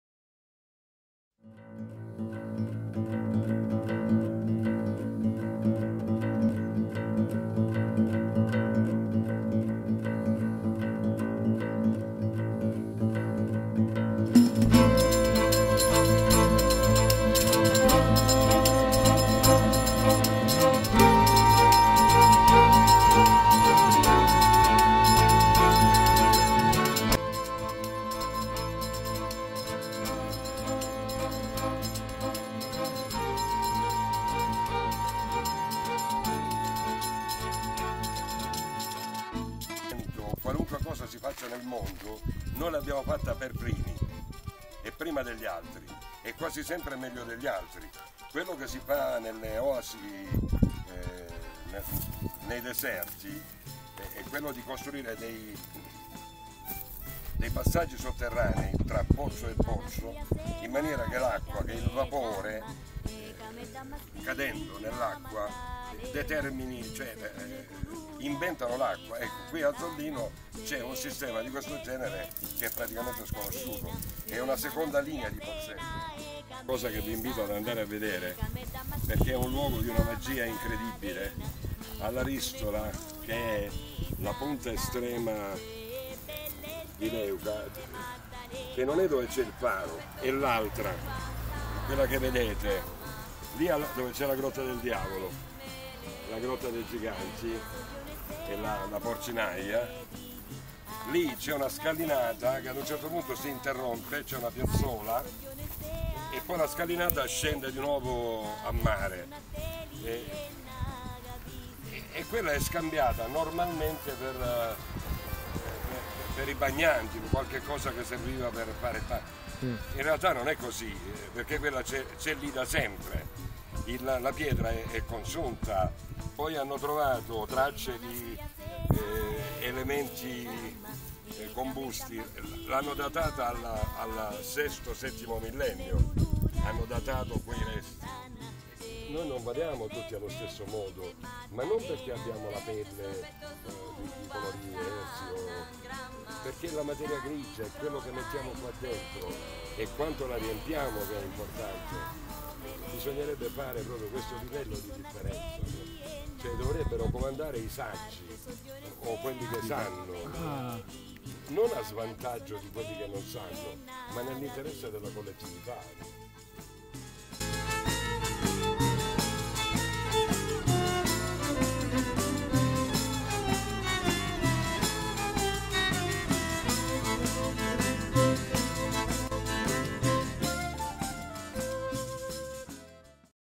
Passeggiata Megalitica - Agosto 2013
Una gita tra Dolmen e Menhir del Salento, con la guida del prof. Alberto Signore, presidente dell'Associazione Amici dei Menhir.
Vi aspettiamo alle prossime passeggiate culturali!